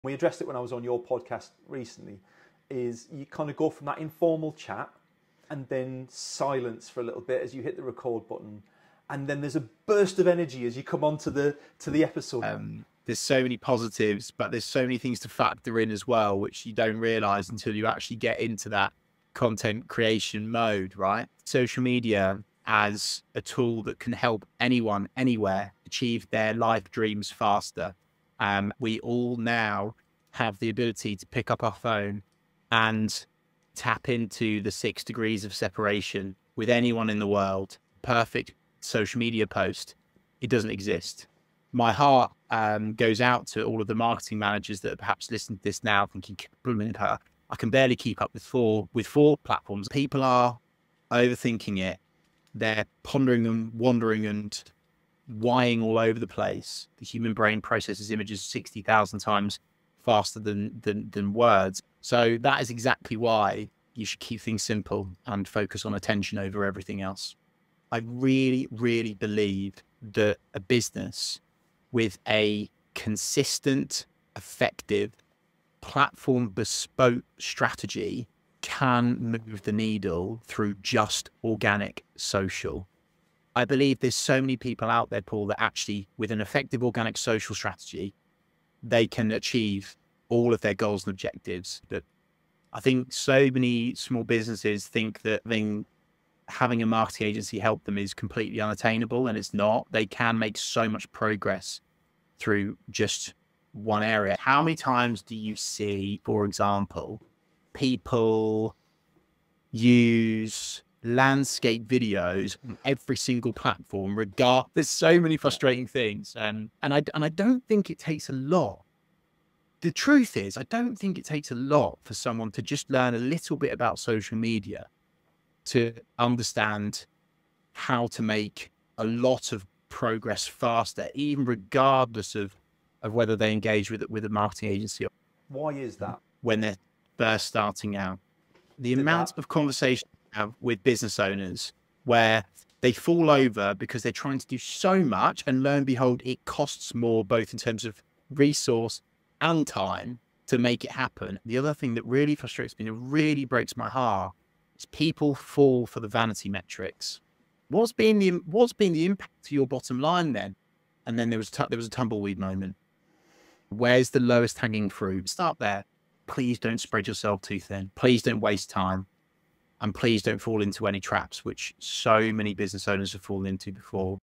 Highlights - The Secret Sauce of Social Success
In this week's episode of MarketPulse, Pros and Pioneers, host Paul is joined by Chris Reeve, founder of Reeve Social Media. Chris's journey into the world of social media began as a side hustle, and over the years, he has managed over 250 social media accounts, including high-profile celebrities. His passion for social media stems from a deep-seated desire to help others achieve their dreams faster through effective social media strategies.

Throughout the episode, Chris shares invaluable insights into managing multiple social media accounts, emphasizing the importance of self-care and consistency. He discusses the significance of knowing your "why" and how it fuels success in social media marketing. Chris also reveals his tips for keeping social media simple, including the powerful content pyramid strategy and the importance of writing engaging, value-driven content.

Listeners will learn why Chris has chosen to specialize in organic social media, steering clear of paid social. He explains the benefits of focusing on what works and avoiding common pitfalls like spreading yourself too thin and falling for vanity metrics. Chris's advice on creating conversations and responding to comments highlights the critical role of engagement in building a successful social media presence.

Don't miss this episode filled with actionable tips and strategies to elevate your social media game.